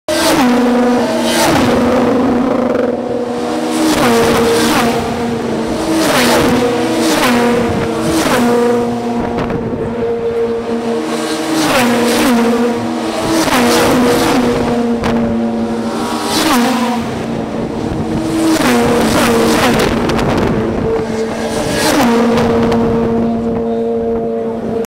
WTCC
WTCC - 2ª corrida. Captei estas imagens na descida da Est. Int. da Circunvalação, com uma pequena máquina fotográfica Casio Exilim EX-Z 750. Os mais rápidos passam aqui a cerca de 250 km/h